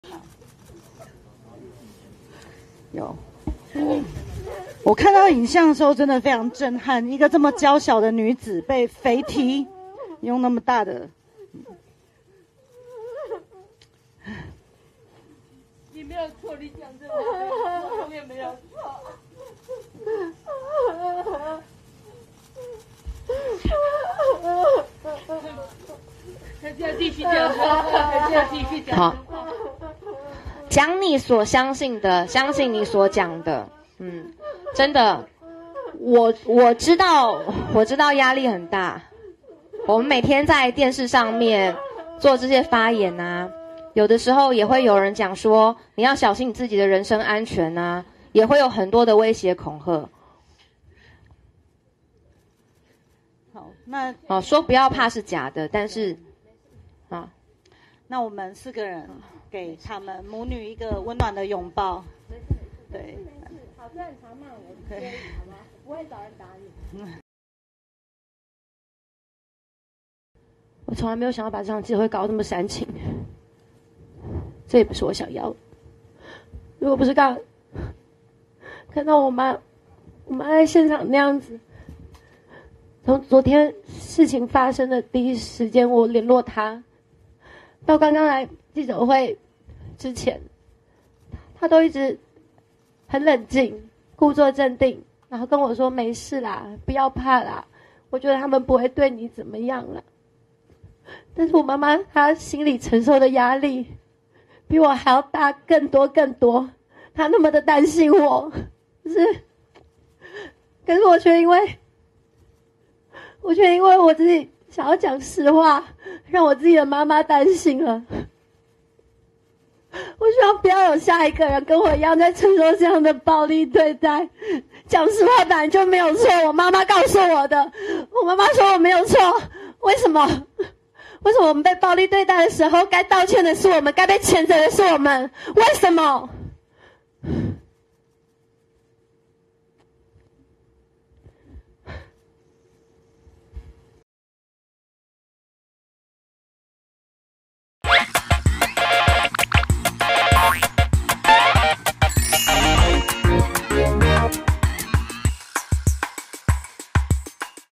陳沂記者會失控淚崩：講實話本來就沒錯
陳沂失控哭嚎：講實話本來就沒錯！　轟超哥施暴亂象「造成模仿」